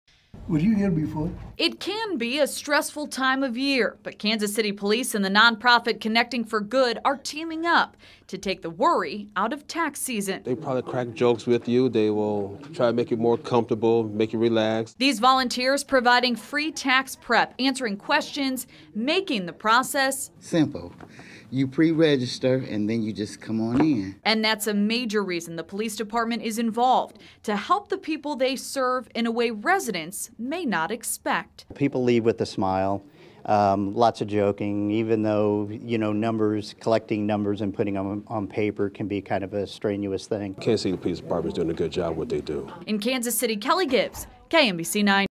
KC police, Connecting for Good offer free tax preparation
Kansas City police and the non- profit group Connecting for Good  are teaming up to provide free tax preparation.